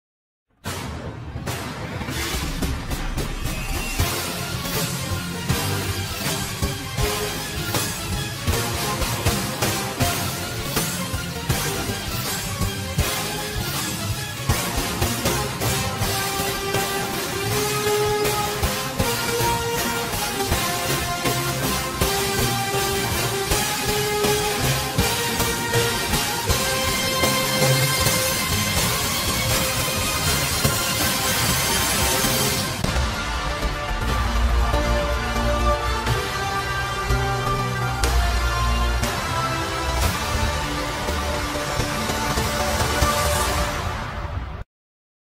The Mizfitz Series Intro (2020 Updated)
May be a small update on this intro, but it's because of a fellow mizfit who will play a big part on our series adventures. sure hope you guys like our series intro 😁